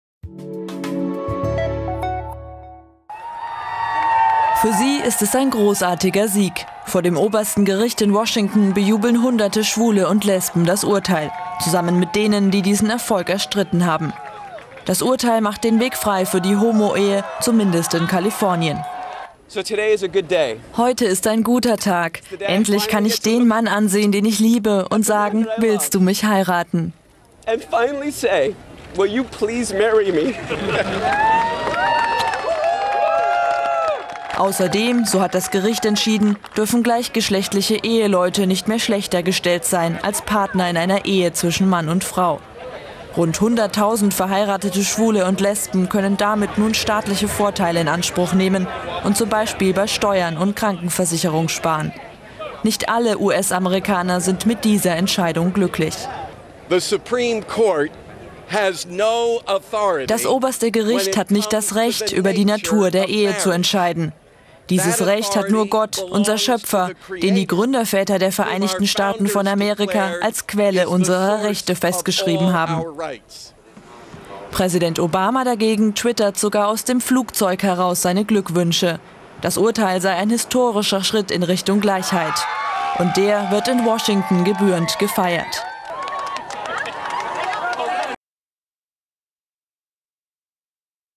USA: Historisches Urteil für Homosexuelle
Der Oberste Gerichtshof der USA hat mit einer bahnbrechenden Entscheidung den Stand der Homo-Ehe gestärkt: Der Supreme Court kippte ein Bundesgesetz, das nur traditionellen Ehen von Mann und Frau Vorteile bei Steuern und Erbschaften gewährte. Die Richter ebneten auch den Weg für die Wiedereinführung der Homo-Ehe in Kalifornien.